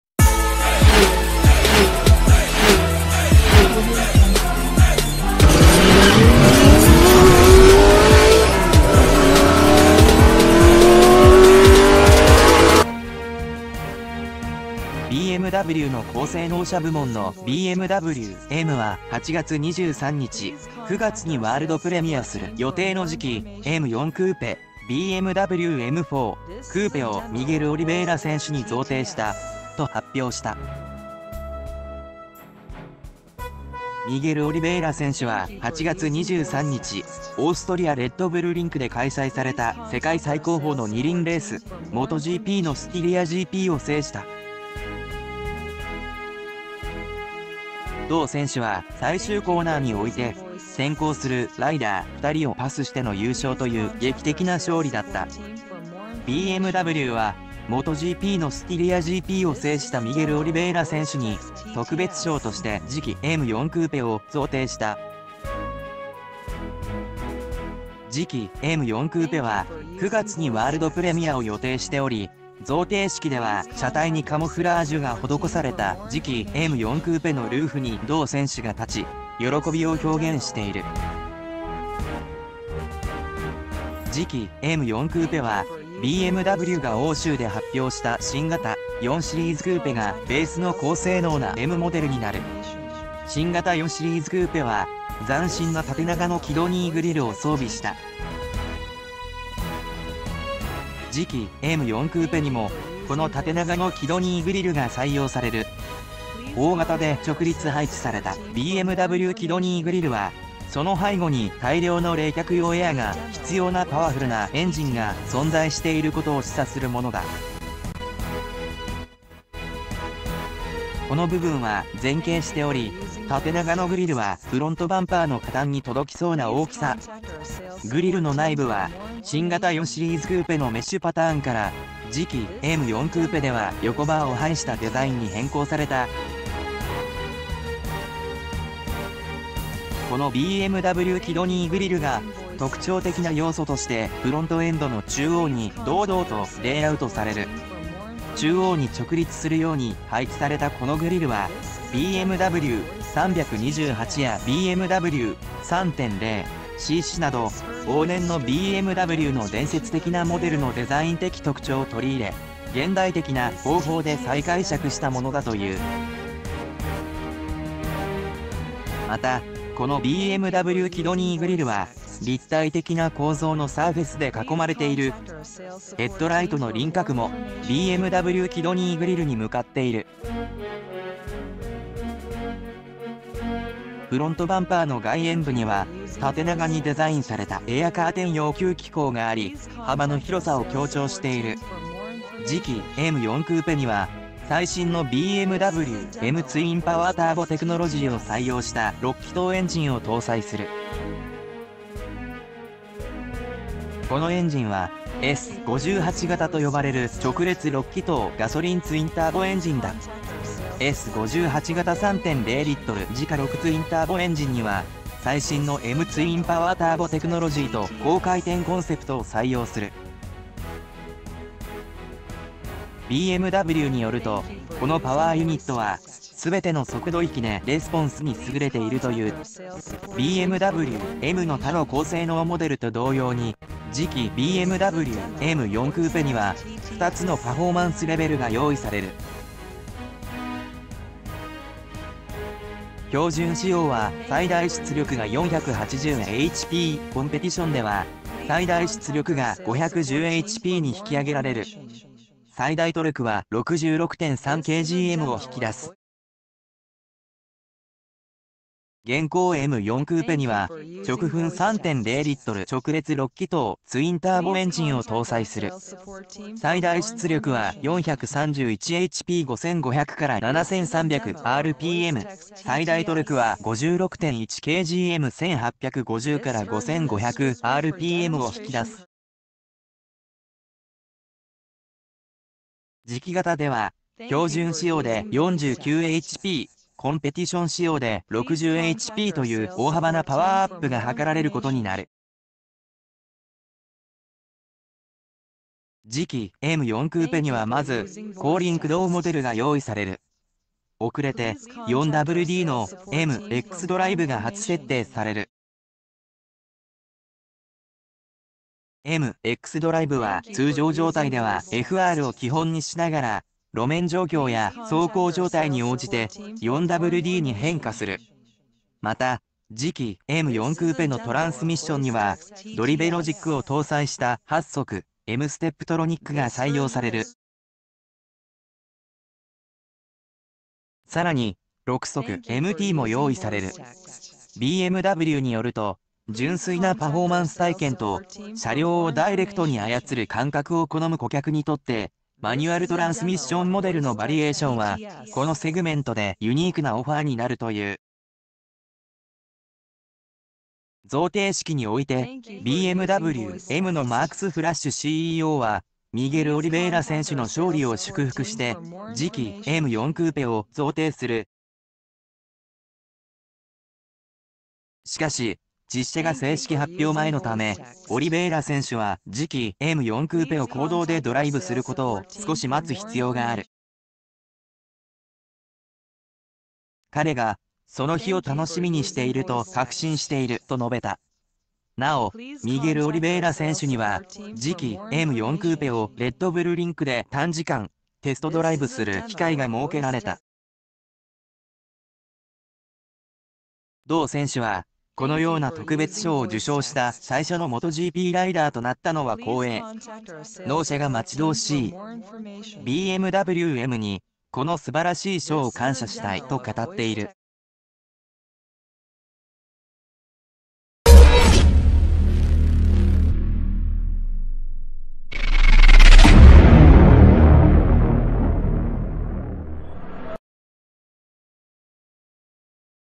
BMW M4クーペ 次期型、MotoGP制したオリベイラ選手に贈呈…9月の公式発表前に ）
,BMW 4シリーズ（BMW 4 Series）,
BMWの高性能車部門のBMW Mは8月23日、9月にワールドプレミアする予定の次期『M4クーペ』（BMW M4 Coupe）をミゲル・オリベイラ選手に贈呈した、と発表した。 ミゲル・オリベイラ選手は